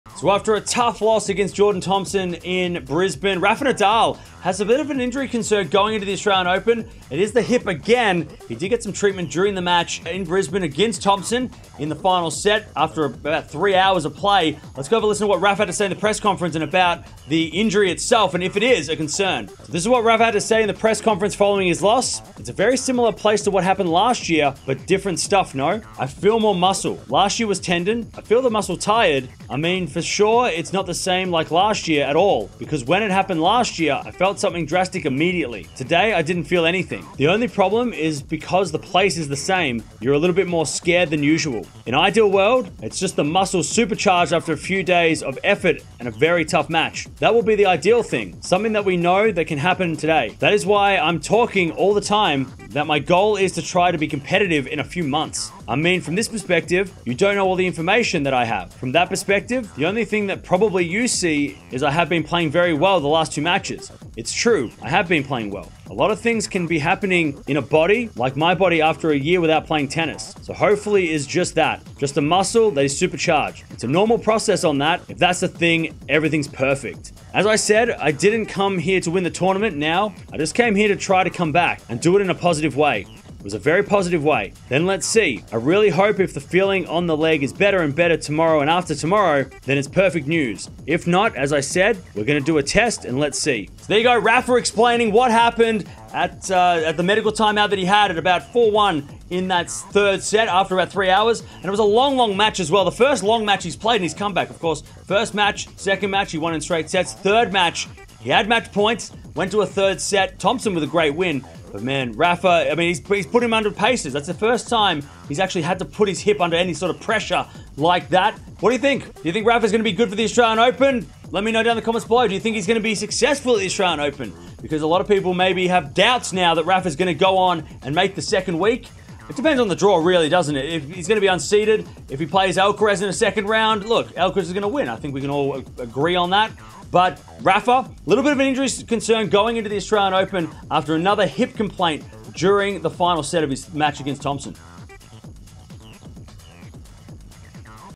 Nadal Injury ahead of Australian Open 2024 | Tennis News
Rafael Nadal has had an injury scare after losing to Jordan Thompson at the Brisbane International 2024. He had to get a medical timeout on his hip in the final set but has said it is not the same injury as last year. How will Rafa go at the Aus Open this year?

0:00 | Intro
0:19 | Nadal Interview
1:33 | Outro